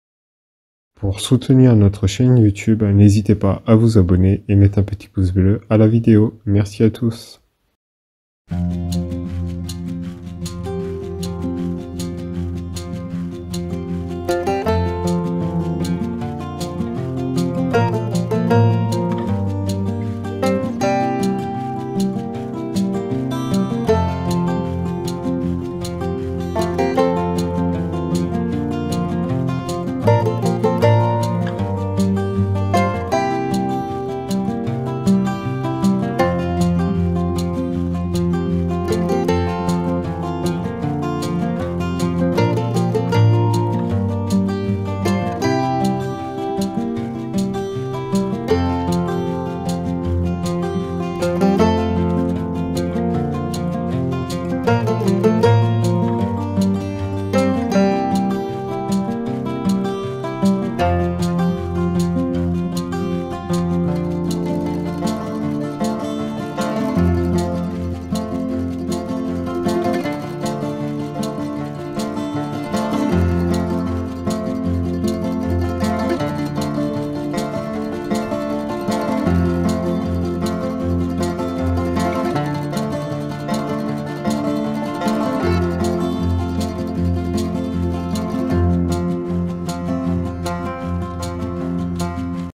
🍅 Gaspacho Supra Rapide – Une Soupe Fraîche en Un Instant
Le gaspacho supra rapide est une soupe froide pleine de fraîcheur et de saveurs, idéale pour l'été. 🍅❄️ Préparez cette recette ultra-simple pour un rafraîchissement instantané. Parfaite pour un repas léger ou un apéritif entre amis. Suivez notre recette rapide pour savourer cette soupe légère !